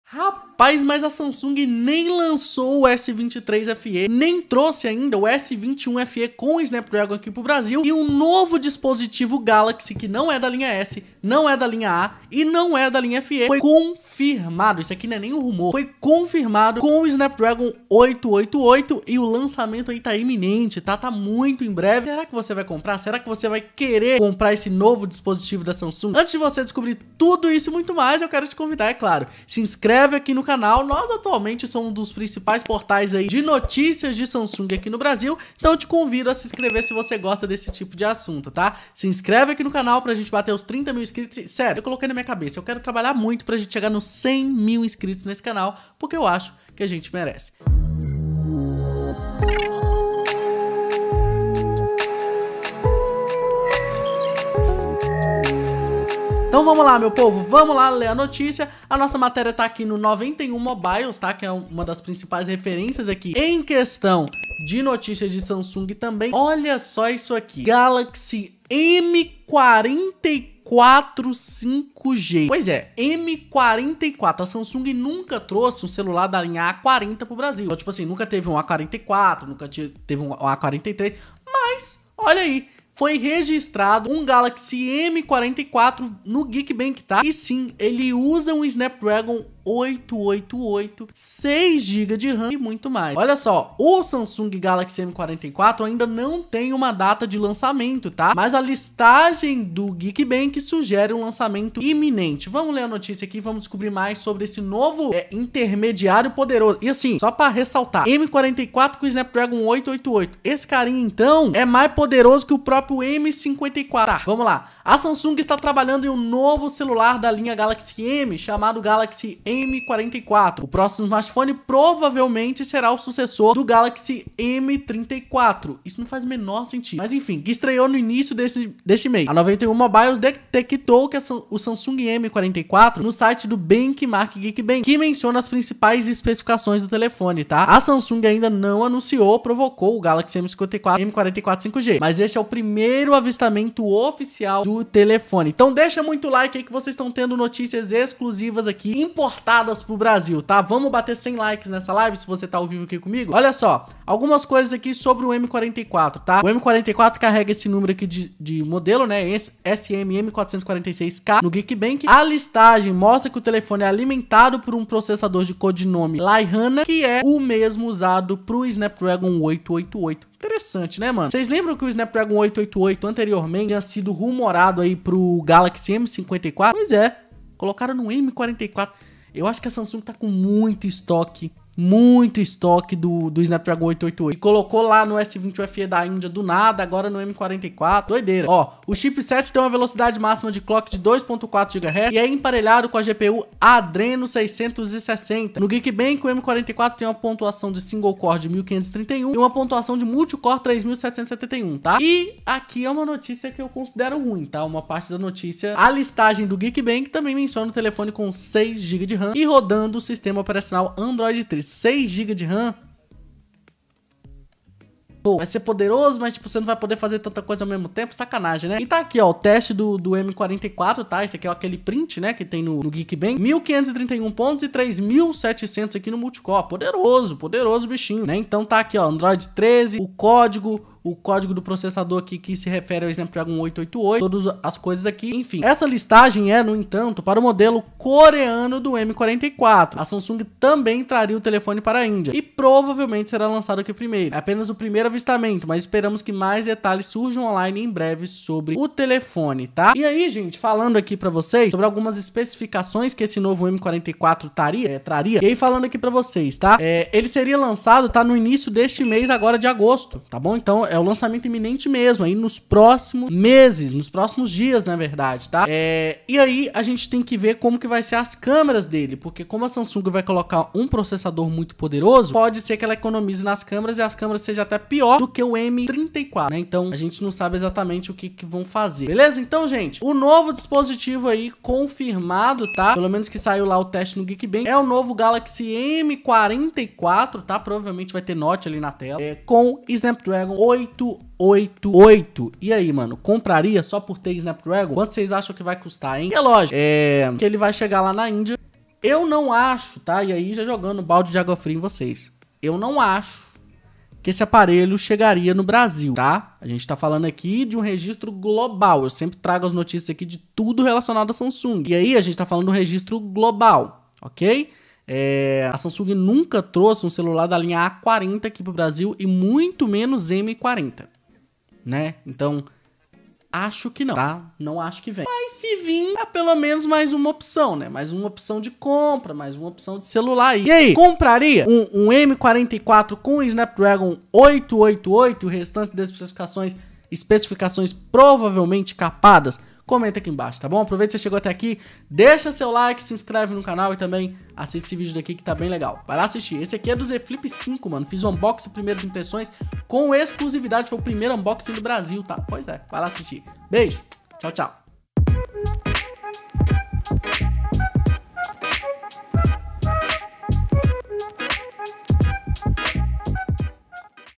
🚨Novo SAMSUNG com Snapdragon 888 (Não é um Galaxy FE)
🚨LANÇAMENTO! Novo SAMSUNG com Snapdragon 888 (Não é um FE)